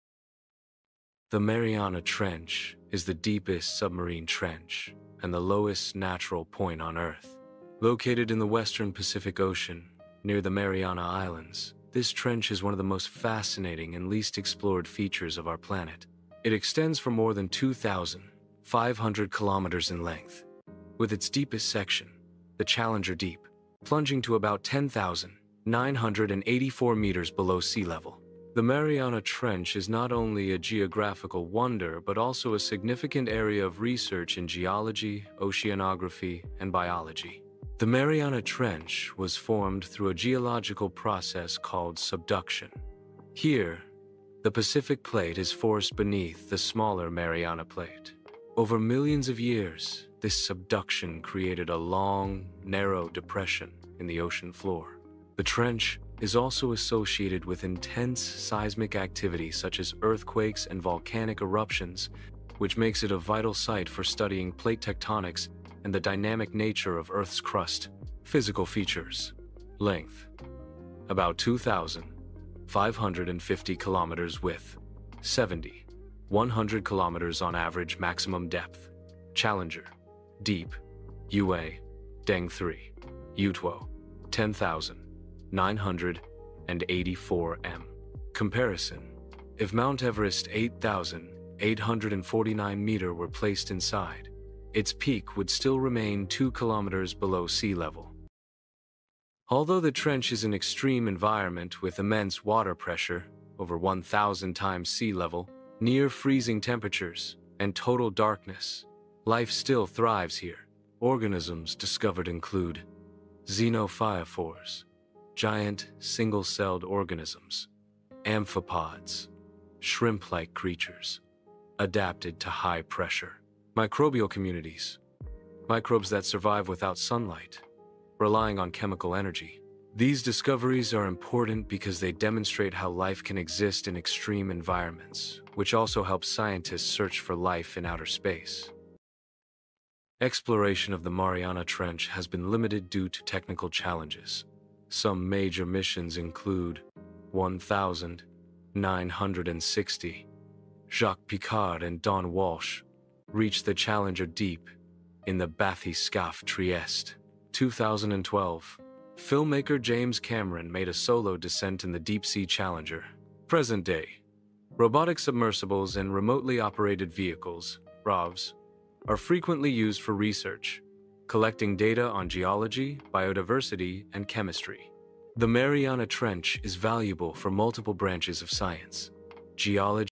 tranche of sea
Dive into the depths of **world geography** as we explore the mysterious **Mariana Trench**—the deepest part of our oceans! In this captivating journey through **sea geography**, uncover fascinating facts about this underwater marvel, from its extreme depths to the unique species that inhabit its pitch-black waters. How does the Mariana Trench impact our planet's geology, and what secrets lie within? Join us for stunning visuals, expert insights, and a deep dive into one of Earth's final frontiers. 🌍🐋